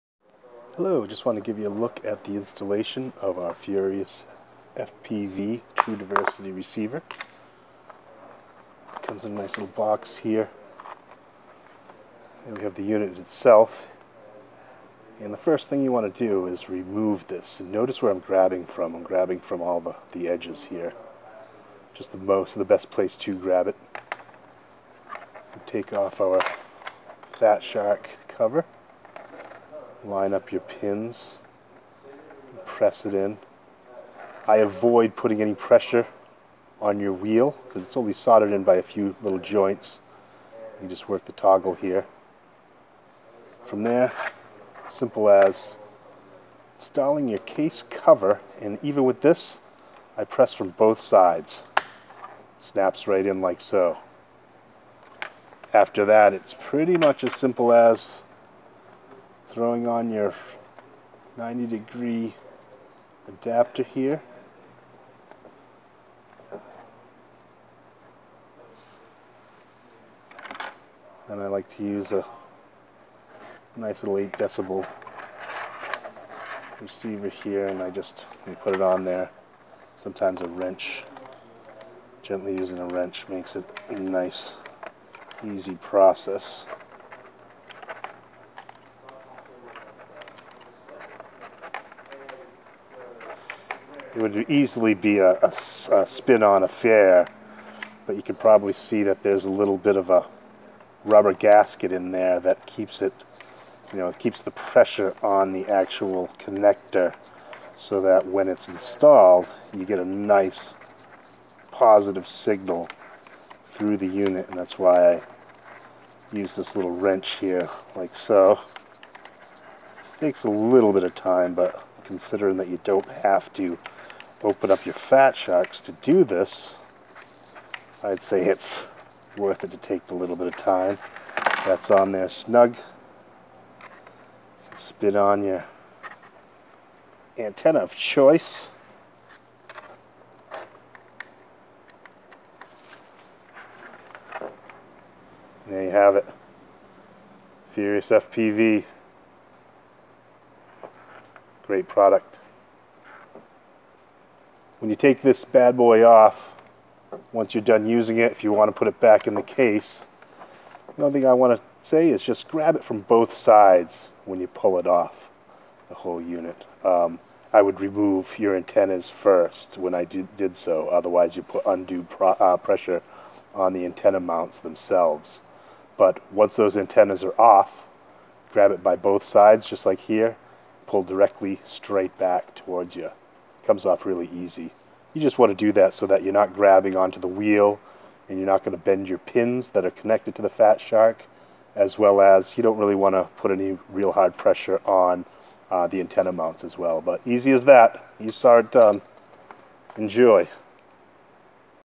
True Diversity Receiver Installer
Here's a simple installation process for the new  FuriousFPV True Diversity Receiver.